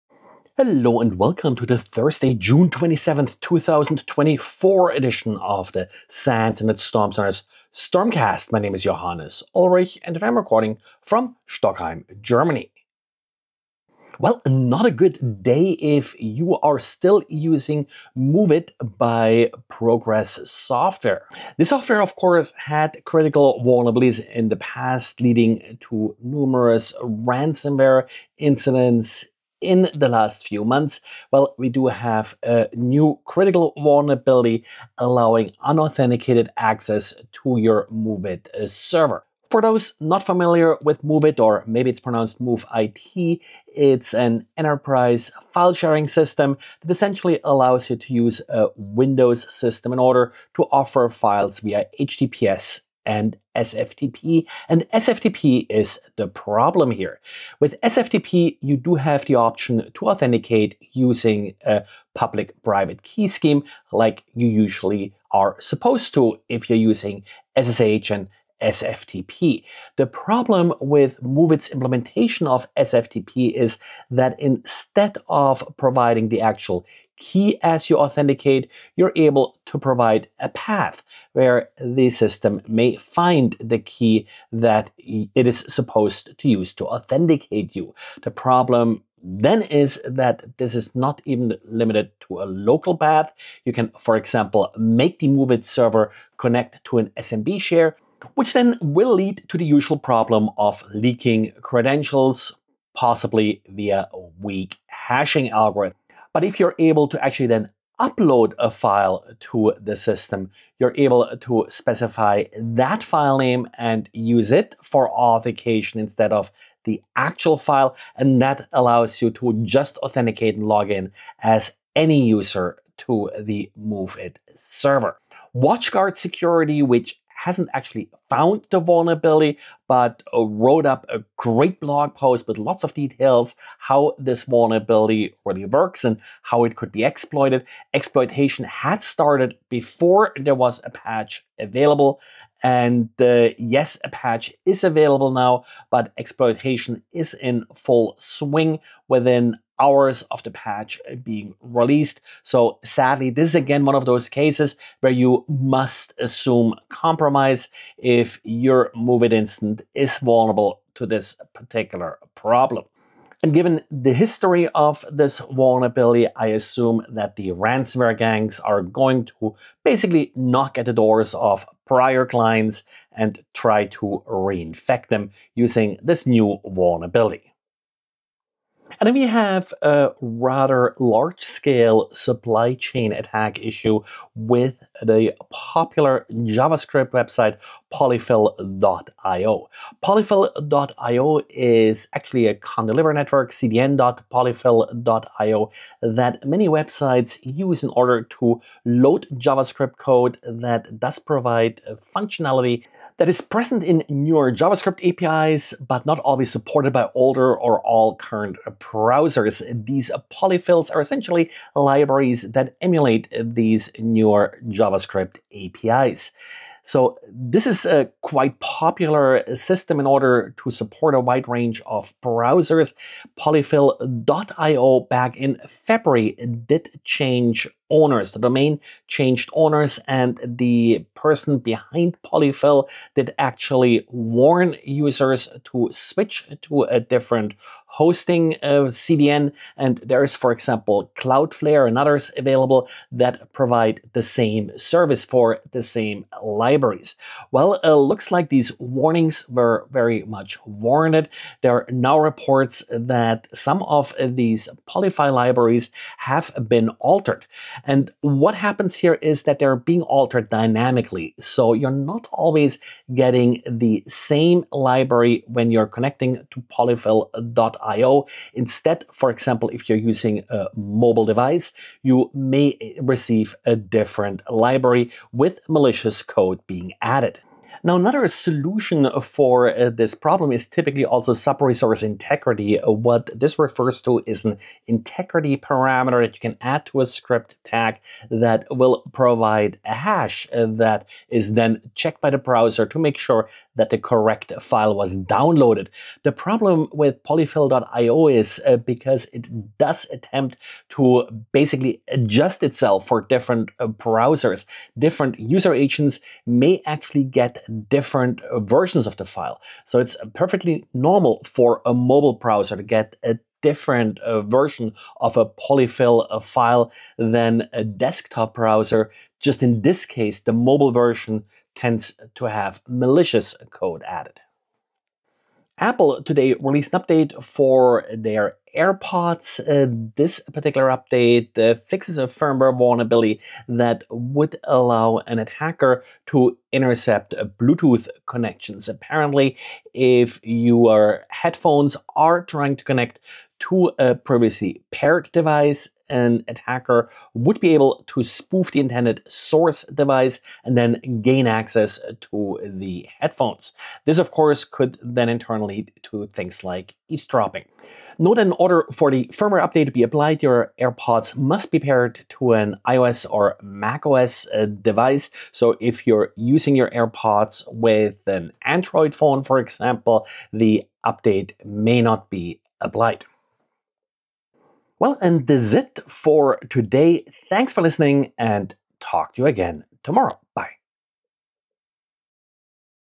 Network Security News Summary for Thursday June 27th, 2024
New MOVEit Vulnerability; Polyfill Supply Chain Attack; Apple AirPods Patch;